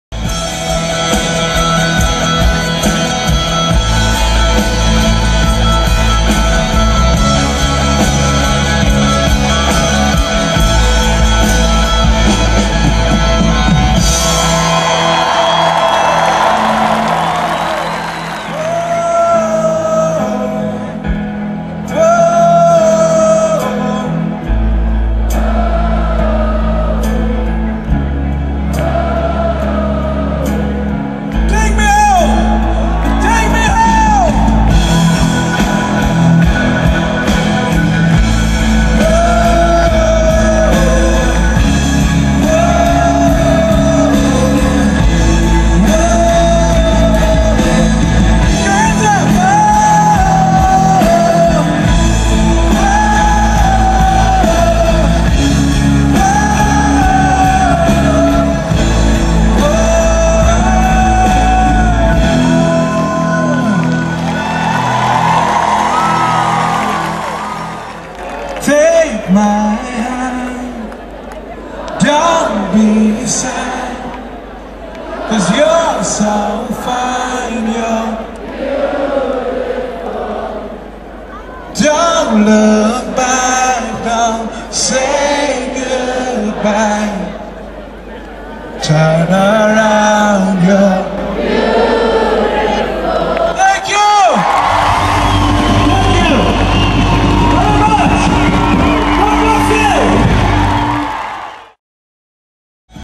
Winter Moods LIVE supporting Bryan Adams
Winter Moods performing live during Bryan Adams's concert in Malta